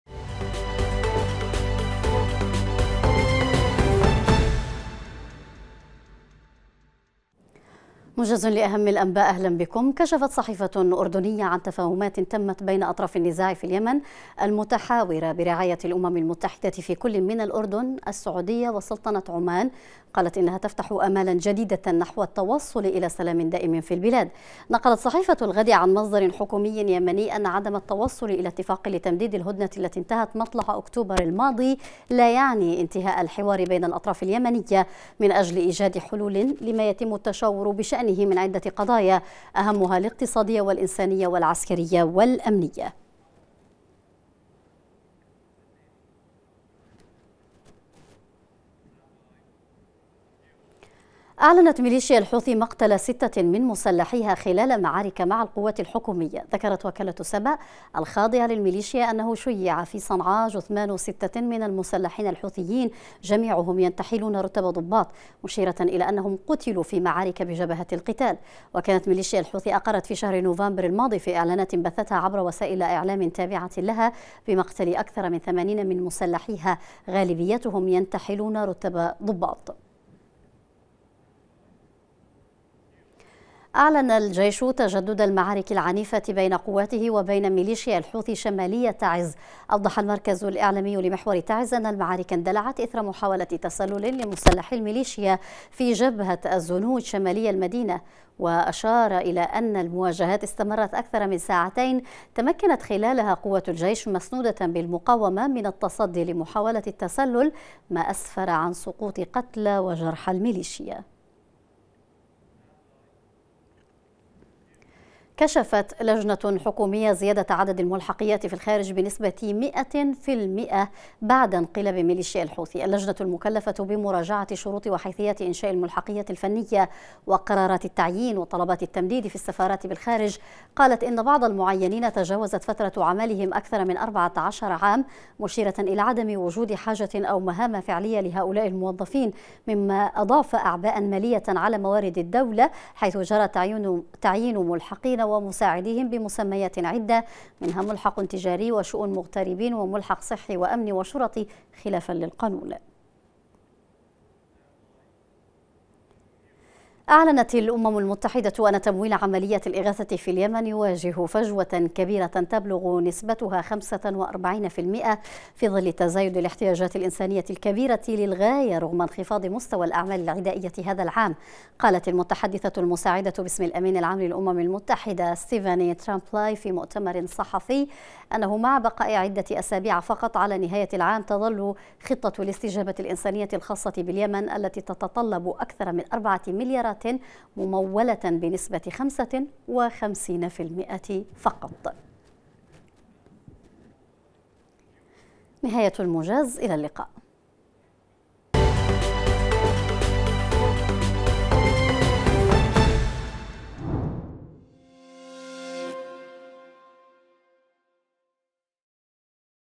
موجز | 06 - 12 - 2022 | 16.00
الأزمة اليمنية
صحيفة أردنية تكشف عن تفاهمات تمت بين أطراف النزاع في الأردن والسعودية وعُمان

جبهات القتال
مليشيا الحوثي تعلن مقتل 6 من مسلحيها خلال معارك مع الجيش

محافظة تعز
تجدد المعارك العنيفة والجيش يعلن التصدي لمحاولة تسلل حوثية في جبهة الزنوج

السلك الدبلوماسي
تقرير حكومي يكشف زيادة عدد الملحقيات بالخارج بنسة 100% منذ 2015